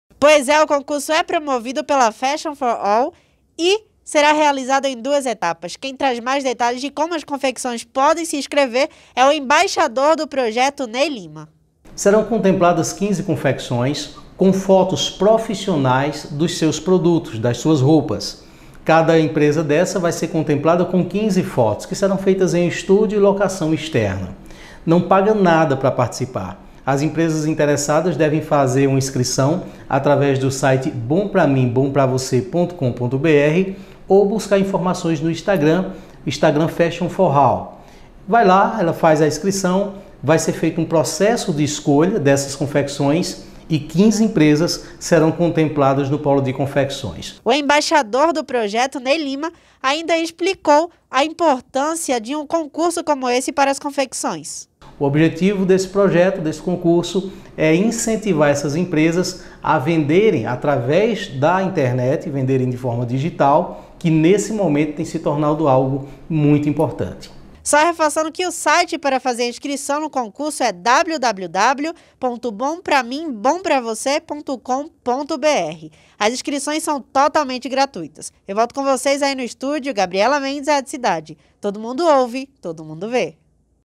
Concurso de moda vai premiar confecções do Agreste - Jornal da Cidade 30/07/2020
No Agreste, será realizado um concurso de moda tendo como premiação um ensaio fotográfico. Se você se interessou em saber mais sobre esse assunto, acompanhe a reportagem de Gabriela Mendes.